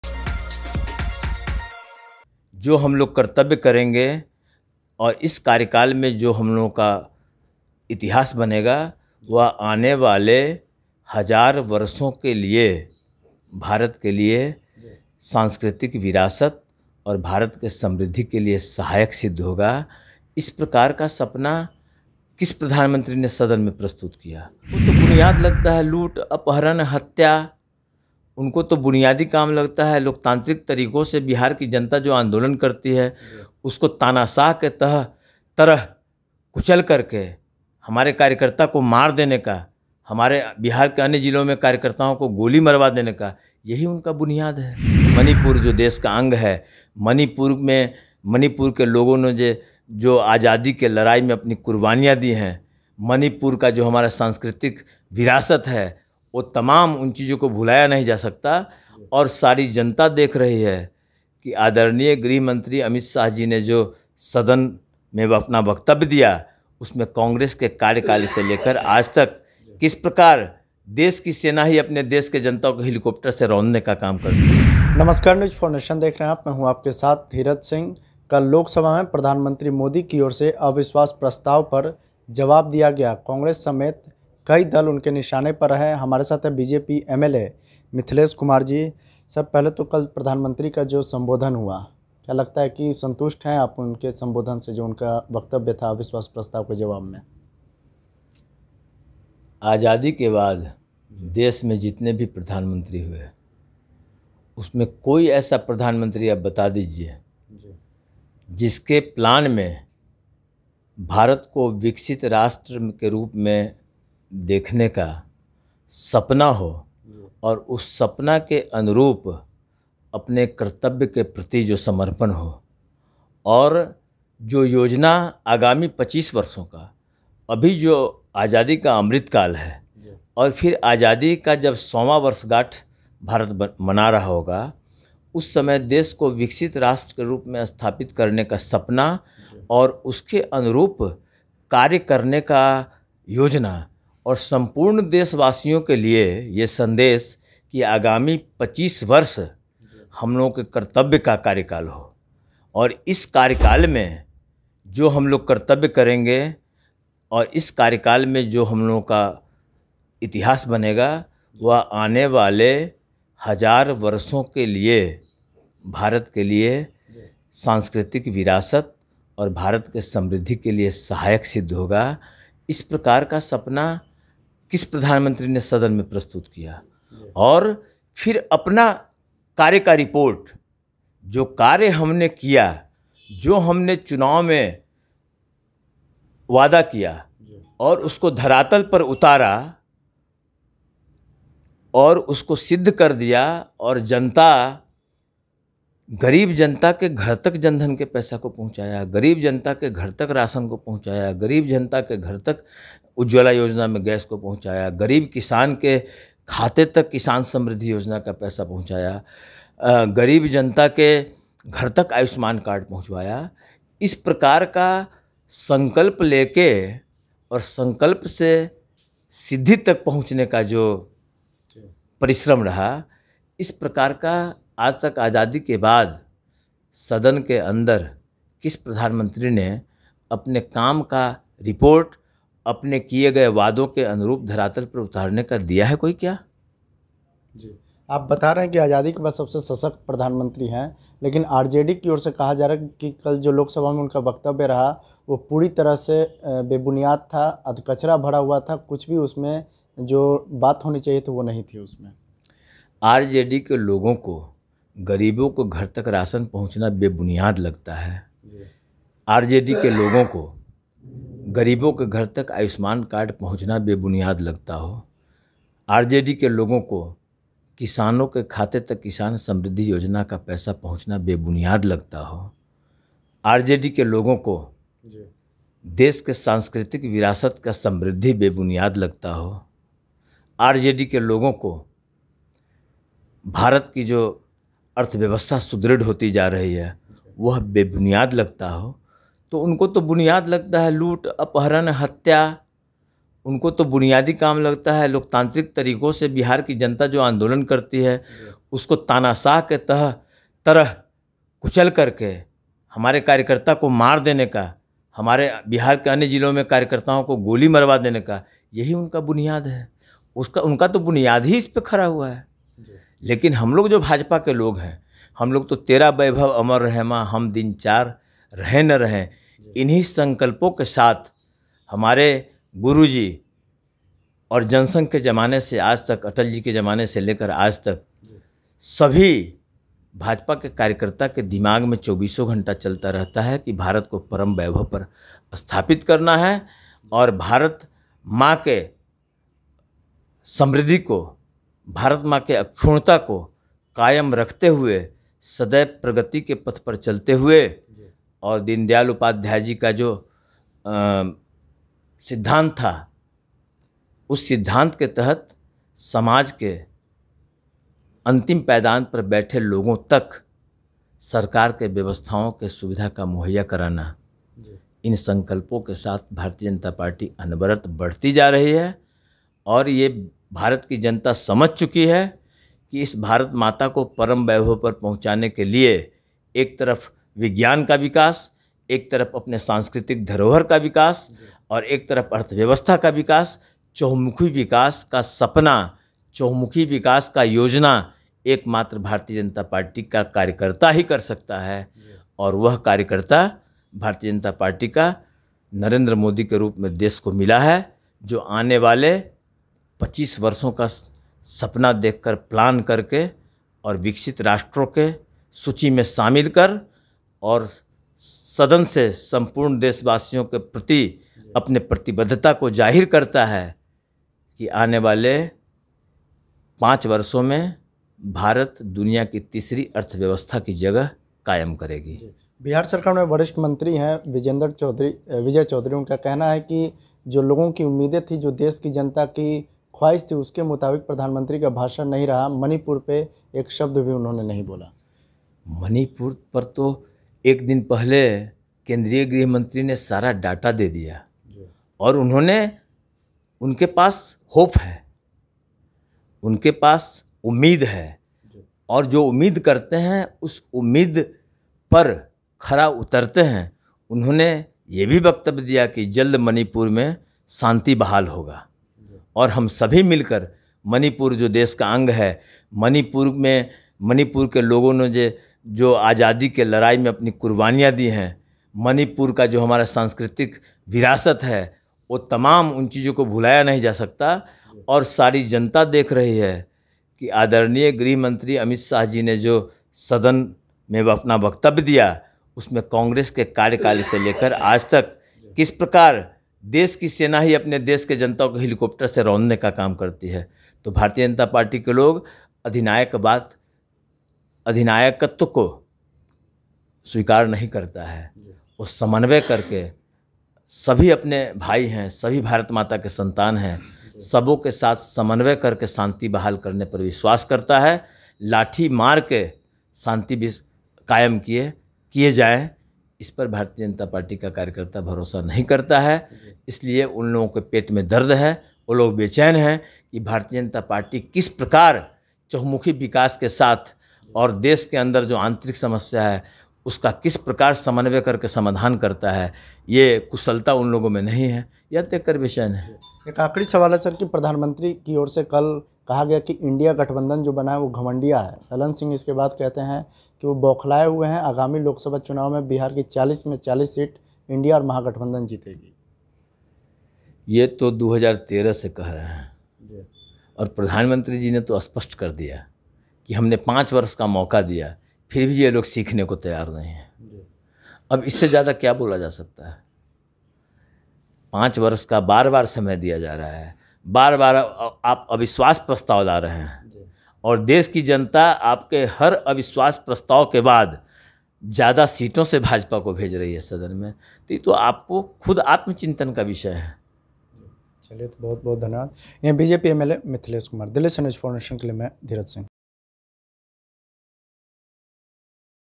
RJD और Lalan Singh पर खूब बरसे BJP के ये MLA, PM Modi के भाषण को बताया ऐतिहासिक
RJD और Lalan Singh पर खूब बरसे BJP के ये MLA, PM Modi के भाषण को बताया ऐतिहासिक | Bihar News| News4Nation